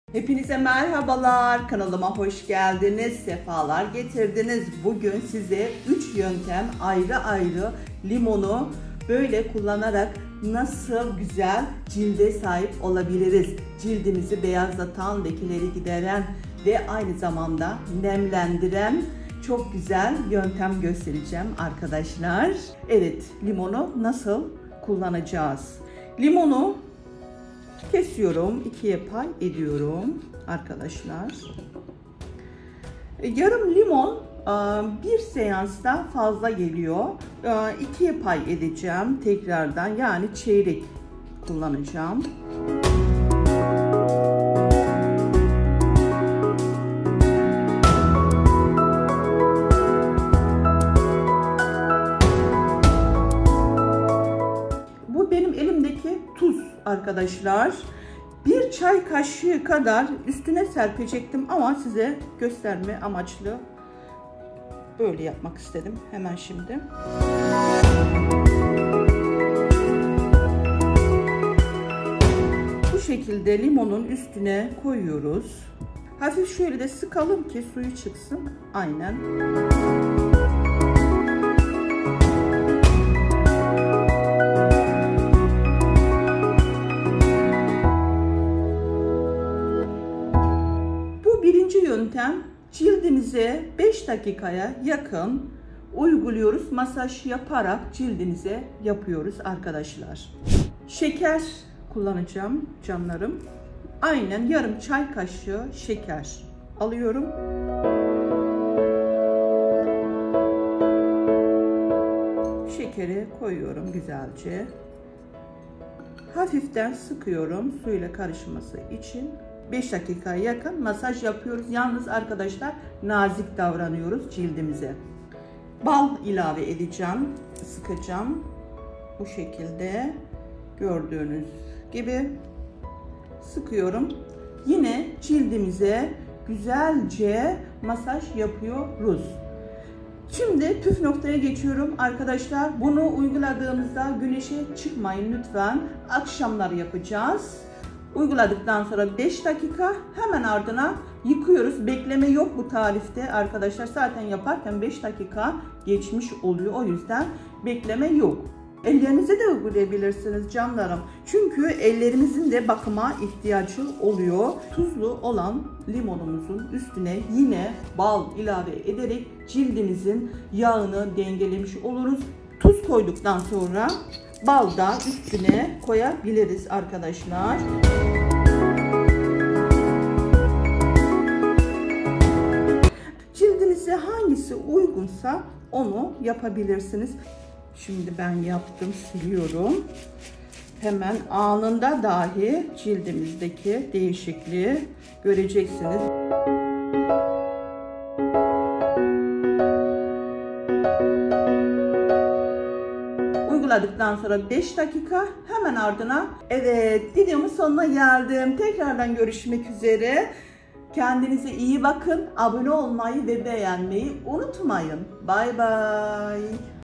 Limonu Böyle Kullanıyor ! 20 Yaşında Görünüyor, Kırışıklık Giderici, Botoks Etkili
Ben Leyla Çevik güzellik bakım uzmanı. limonu böyle kullanıyor ve 20 yaşında görünüyor. bu tarif ile evde doğal botoks ve kırışıklık giderici maske hazırladım. cilt bakım ve yaşlanma karşıtı maske ile cildinizi güzelleştirebilirsiniz. Güzellik, cilt bakım ve maske için beğenmeyi unutmayın.

SEVGİLER...🧿

►VİDEOMA YORUM YAPMAYI İHMAL ETME

NOT: Youtube Kanalım da kişisel bakım videoları hazırlıyorum.Asla Tedavi Amaçlı Değildir.UYGULAMALARDA Maskeleri Kulak Yanın da Veya Bilekte Test Ederek Uygulayın.Alerji Durumu Varsa Yapmayınız.

SİZİ SEVİYORUM :))